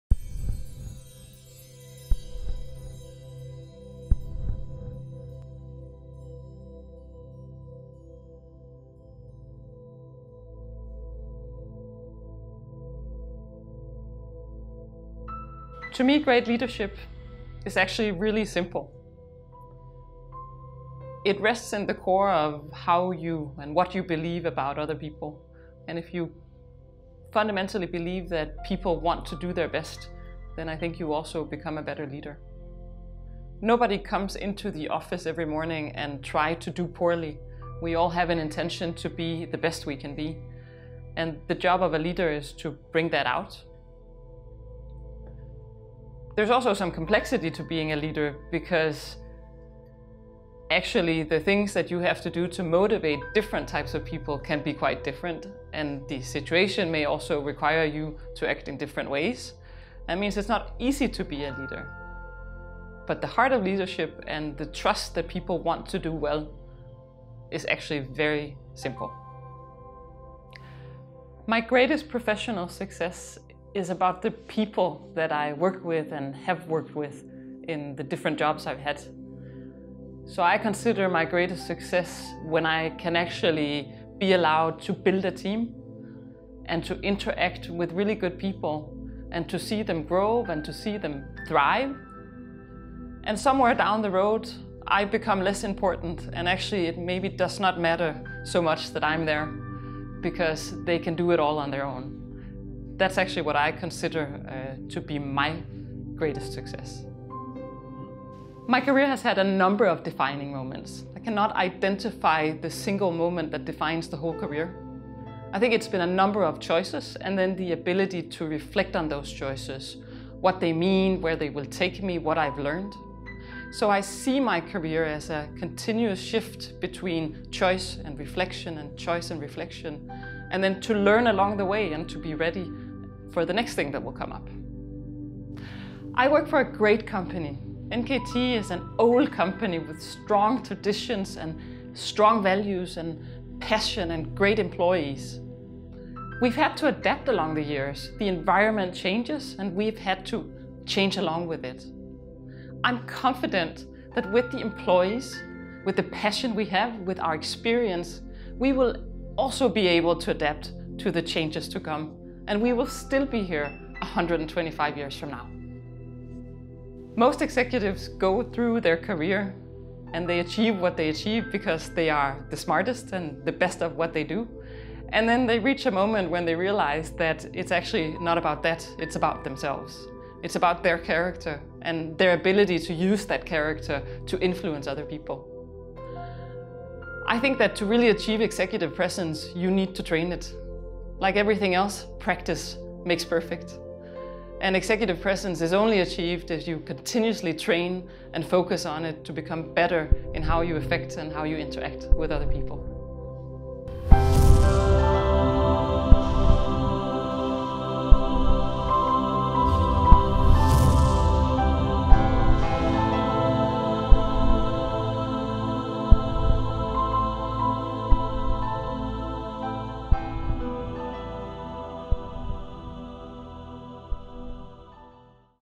CHRO, Executive Vice President Lika Thiesen - NKT - The HR List
CHRO, Executive Vice President Lika Thiesen - NKT talks about Leadership, her greatest success, a defining moment and Executive Presence.